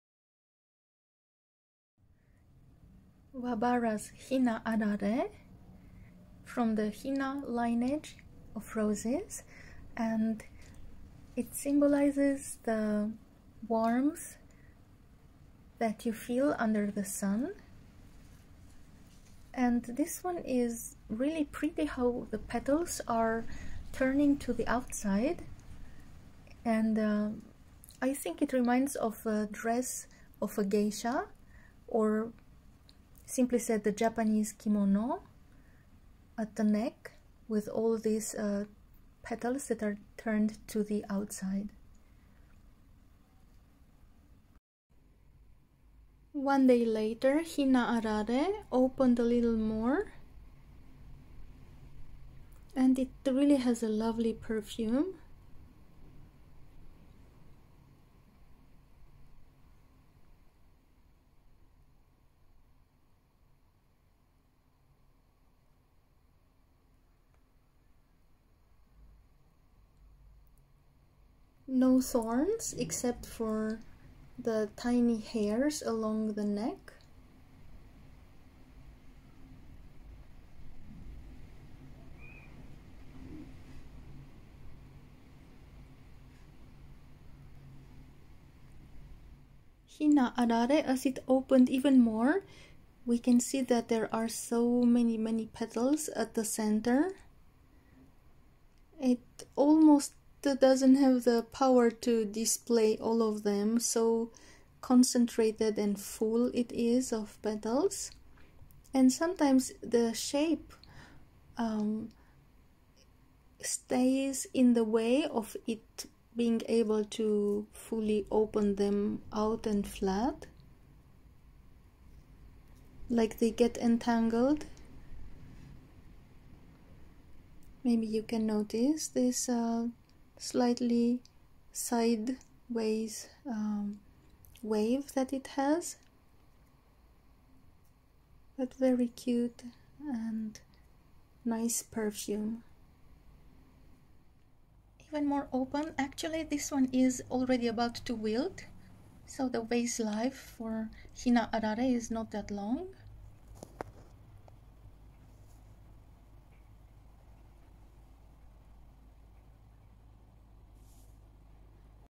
HINAALALE ROSE plant by Keiji Kunieda WABARA Japan 和バラ ひなあられ ワバラ Rose Farm KEIJI
The colors and expressions of a slightly softer doll are put on the innocent girl of the Hinamatsuri.

 Lineage: Hina Series
 Type: single bloom
 Fragrance: faintly fragrant

ひなを少し柔らかくしたような色彩と表情を、ひな祭りのあどけない少女の姿に乗せて。

系統：ひなシリーズ

タイプ：一輪咲き

香り：ほのかに香る